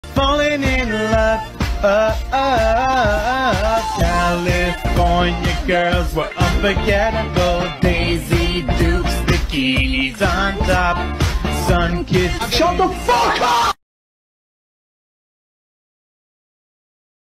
(NOT FOR KIDS!!!) Dee SWEARS!!?! | PBS Kids Gacha Life
Dee was so annoyed of the song that she didn't like that Mario is attacked to it. she yell at the radio but in the wrong way that shocked Sonic and Mario. and at the end Del is calming Dee down while Mario is still singing and Sonic is wondering how did she swears